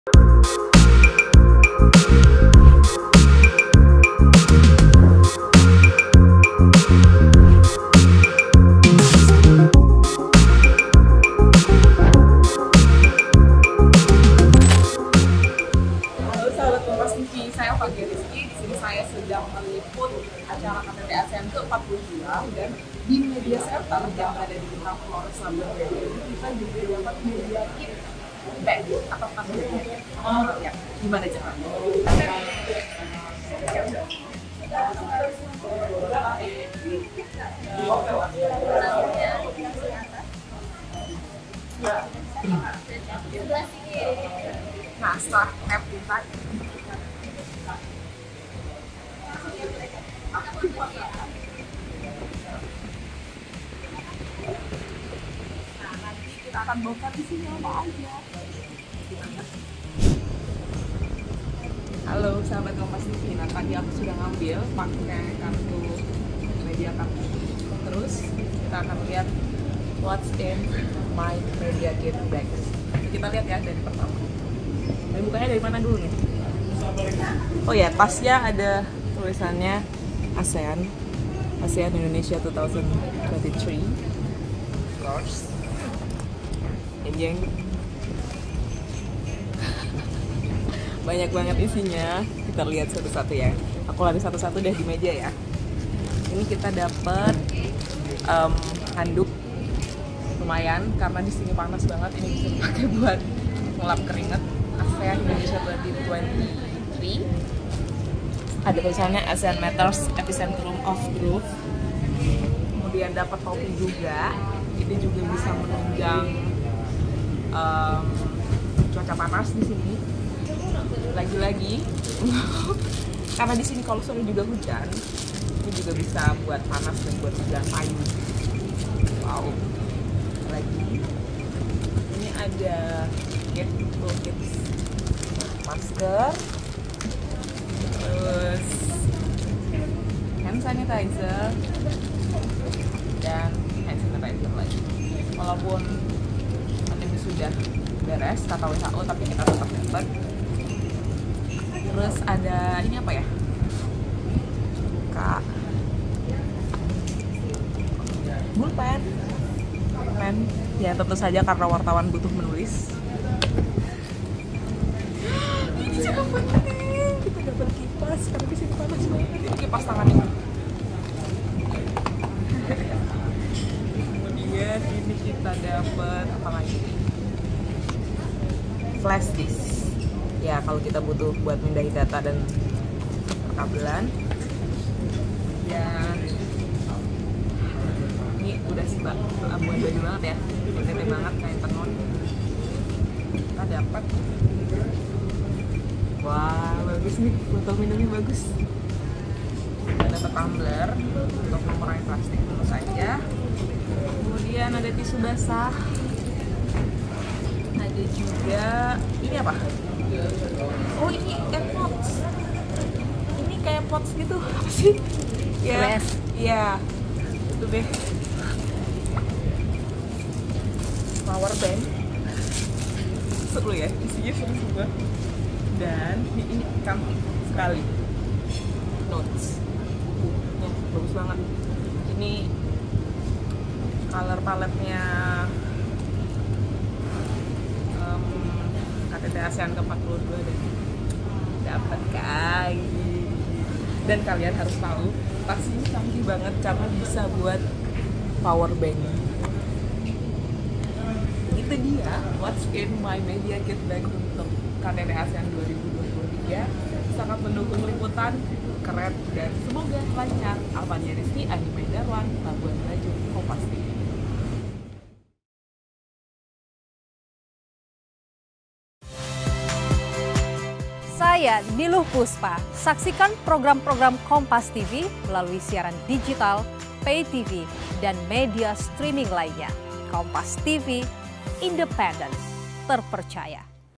KTT ASEAN Labuan Bajo di Depan Mata, Ini Isi Media Kit Para Jurnalis! VLOG
LABUAN BAJO, KOMPASTV - Gelaran KTT ASEAN ke-42 di Labuan Bajo segera berlangsung.

Para jurnalis dari penjuru negeri dan luar negeri sudah mulai merapat ke Labuan Bajo, NTT.

Panitia memberi fasilitas lengkap untuk memenuhi kebutuhan kerja para jurnalis.

Jurnalis KompasTV Alfania Risky bongkar isi media kita untuk sahabat KompasTV.

Tas untuk jurnalis berisikan alat-alat penunjang kerja, seperti alat tulis, powerbank, flashdisk, earphone, dan lain-lain.

KTT ASEAN akan digelar pada 10-11 Mei 2023 di Labuan Bajo.

Presiden Jokowi dan jajaran pun terus bersiap menyempurnakan gelaran konferensi tingkat tinggi se Asia Tenggara ini.

Baca Juga Cek Persiapan Akhir Jelang KTT ke-42 ASEAN, Jokowi: Semuanya Sudah Siap! di